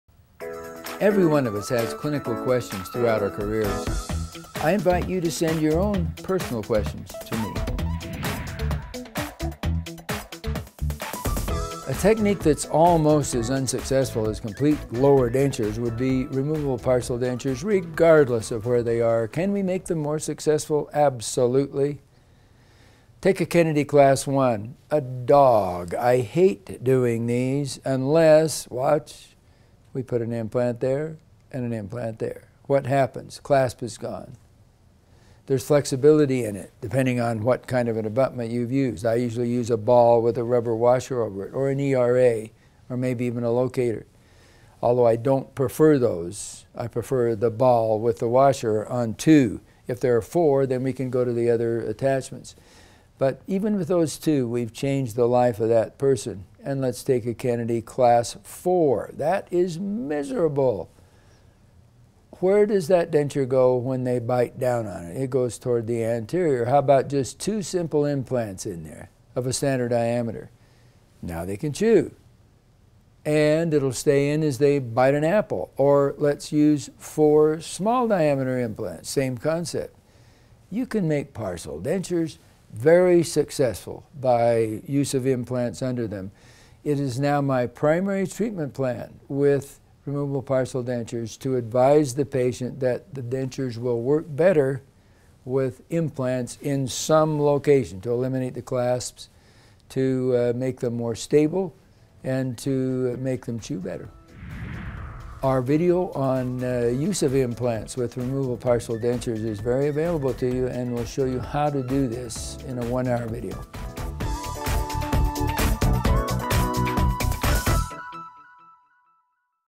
Ask Gordon: Implants and removable partial dentures
Can implants be used successfully with removable partial dentures?